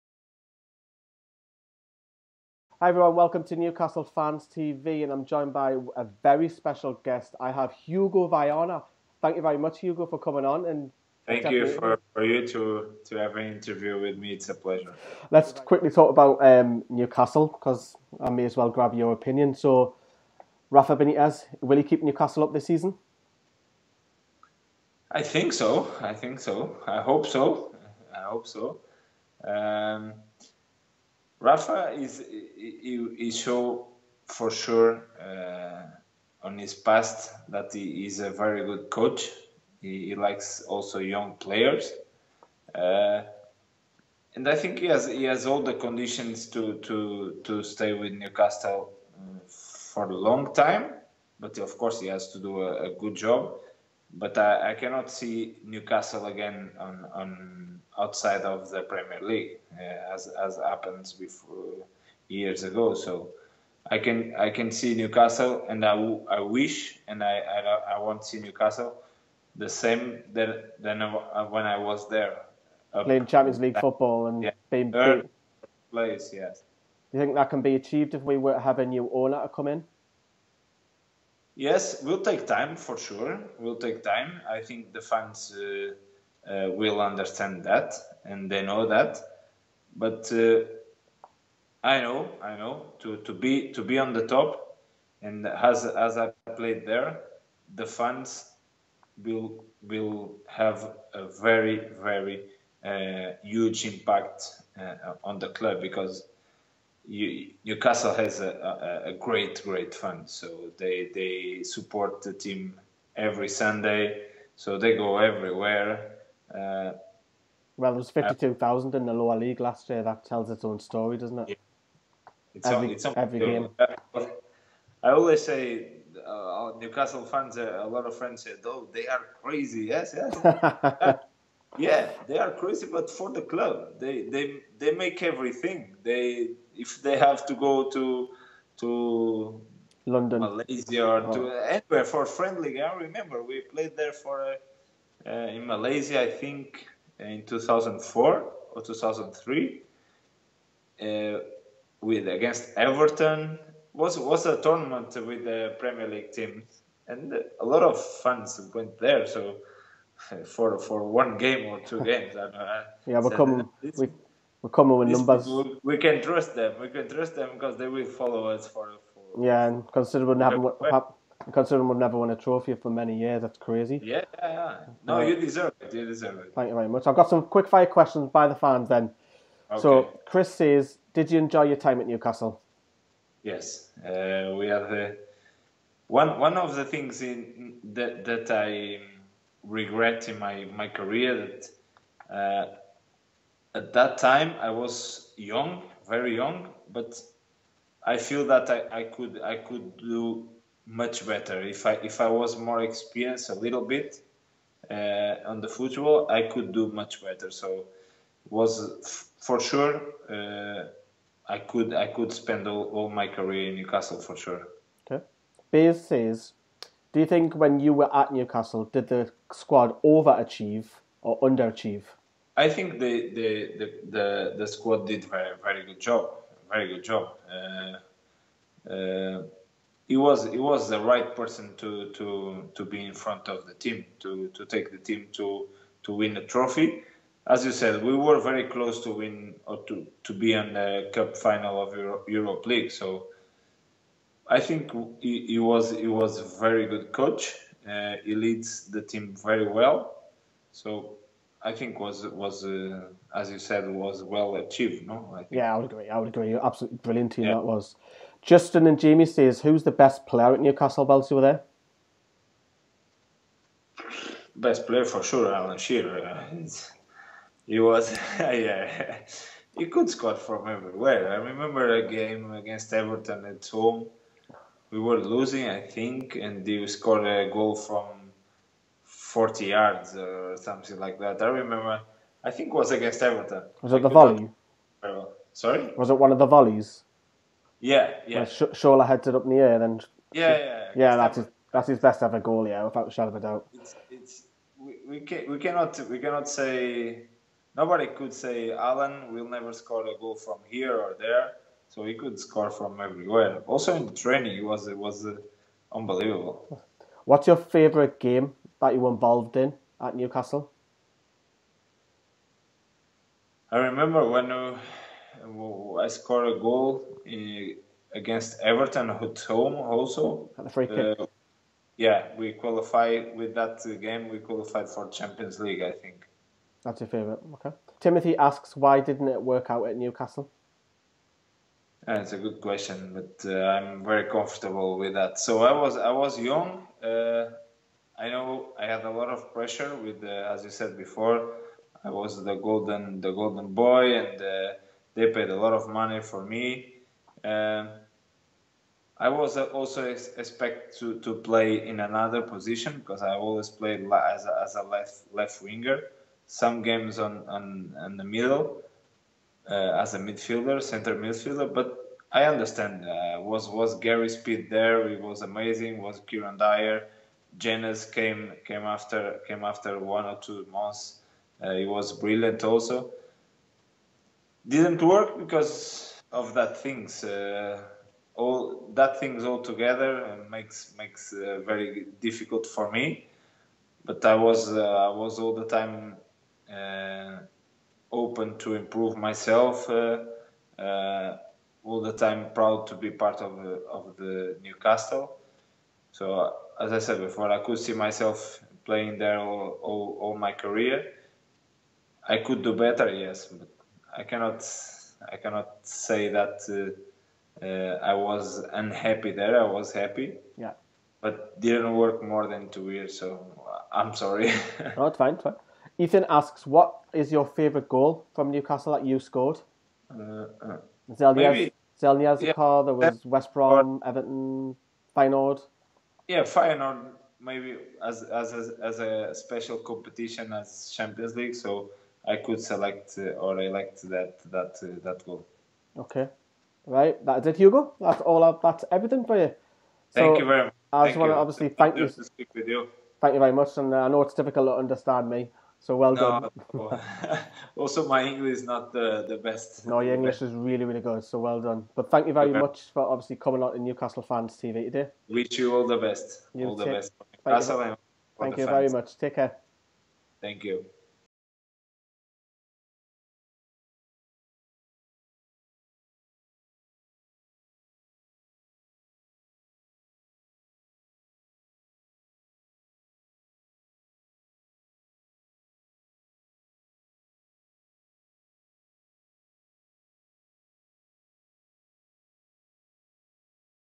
Hugo Viana on Rafa Benitez, Newcastle United & the fans
Hugo Viana chats to Lee about Newcastle, Rafa Benitez & answers some of your questions you sent in to us.

OUR PARTNERSHIPS